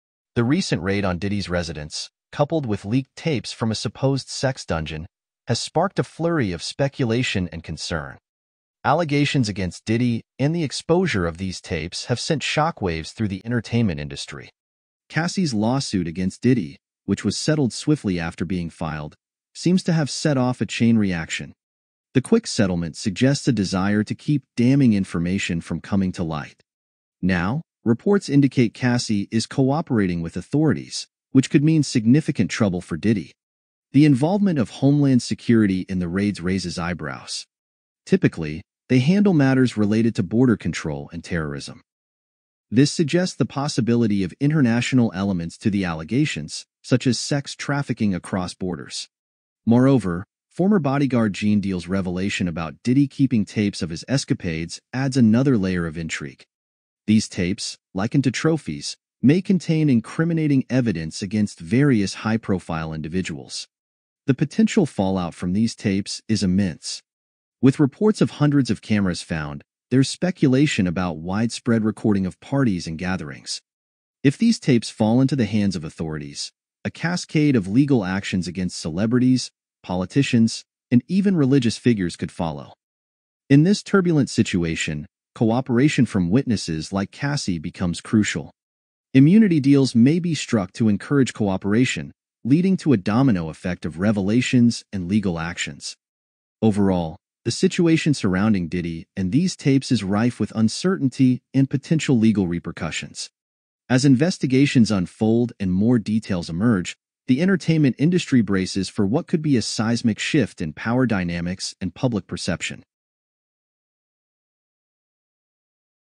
The Feds Just EXPOSED Diddy's FREAKOFF'S With Usher With New footage
The recent raid on Diddy's residence, coupled with leaked tapes from a supposed sex dungeon, has sparked a flurry of speculation and concern. Allegations against Diddy and the exposure of these tapes have sent shockwaves through the entertainment industry.

Cassie's lawsuit against Diddy, which was settled swiftly after being filed, seems to have set off a chain reaction. The quick settlement suggests a desire to keep damning information from coming to light. Now, reports indicate Cassie is cooperating with authorities, which could mean significant trouble for Diddy.

The involvement of Homeland Security in the raids raises eyebrows. Typically, they handle matters related to border control and terrorism. This suggests the possibility of international elements to the allegations, such as sex trafficking across borders.

Moreover, former bodyguard Jean Deal's revelation about Diddy keeping tapes of his escapades adds another layer of intrigue. These tapes, likened to trophies, may contain incriminating evidence against various high-profile individuals.

The potential fallout from these tapes is immense. With reports of hundreds of cameras found, there's speculation about widespread recording of parties and gatherings. If these tapes fall into the hands of authorities, a cascade of legal actions against celebrities, politicians, and even religious figures could follow.

In this turbulent situation, cooperation from witnesses like Cassie becomes crucial. Immunity deals may be struck to encourage cooperation, leading to a domino effect of revelations and legal actions.

Overall, the situation surrounding Diddy and these tapes is rife with uncertainty and potential legal repercussions. As investigations unfold and more details emerge, the entertainment industry braces for what could be a seismic shift in power dynamics and public perception.